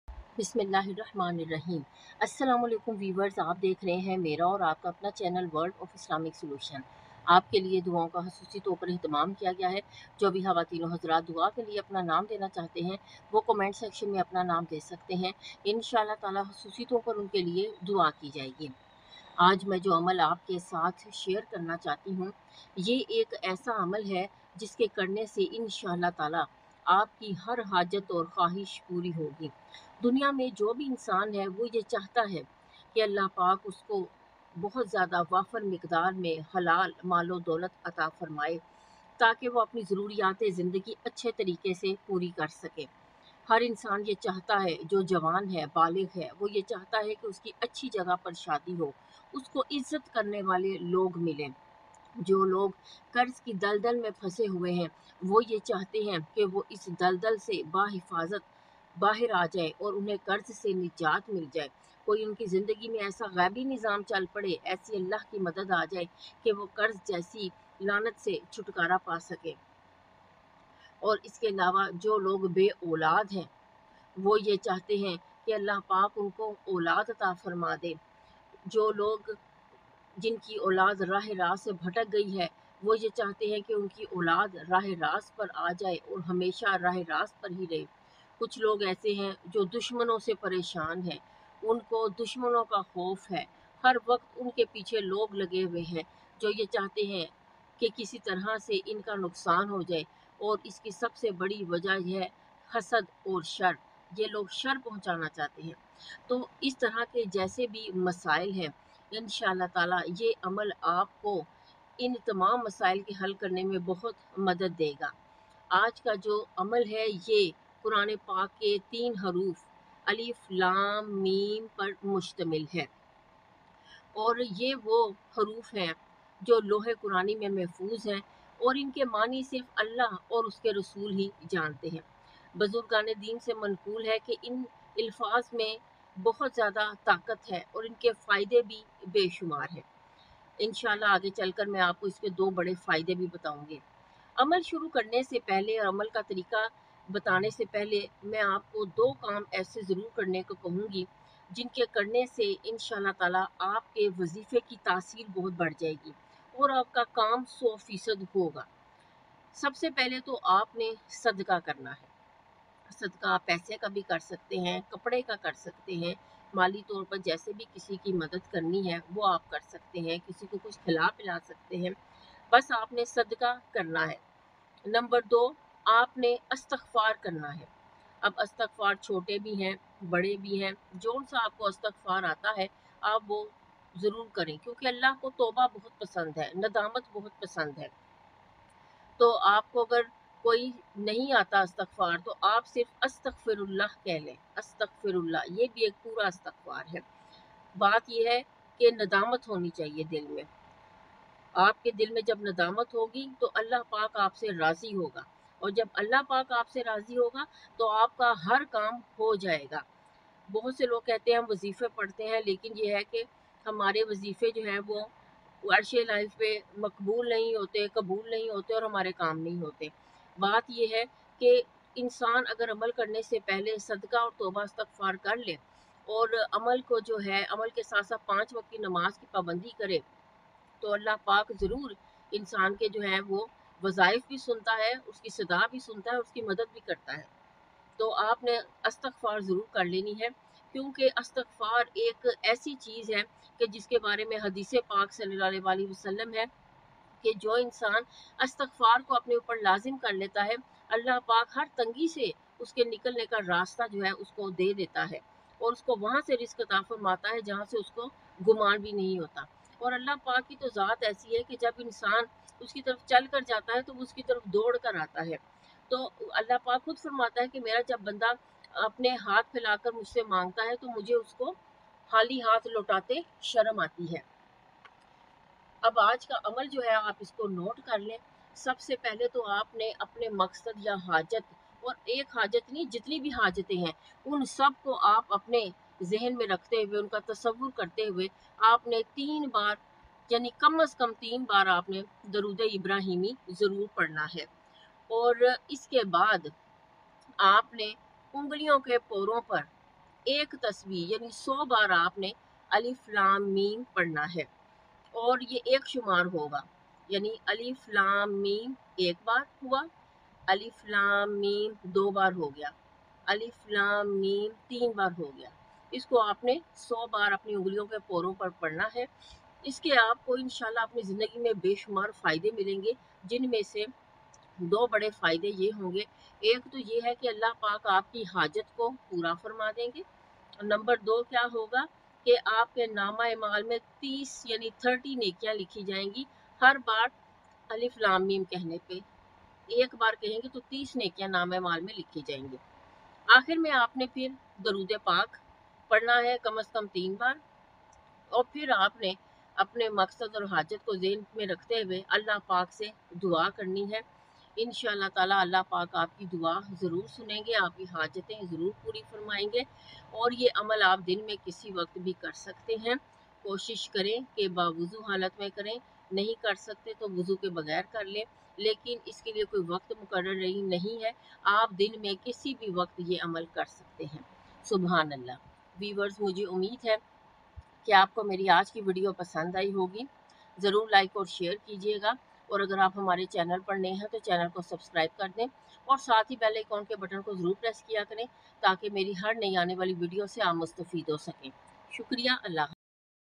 Wazifa of Quran Word tobIncrease Money, Age etc
@worldofislamicsolution2807 In this video amal of Alif Laam Meem is explained to increase money, age etc.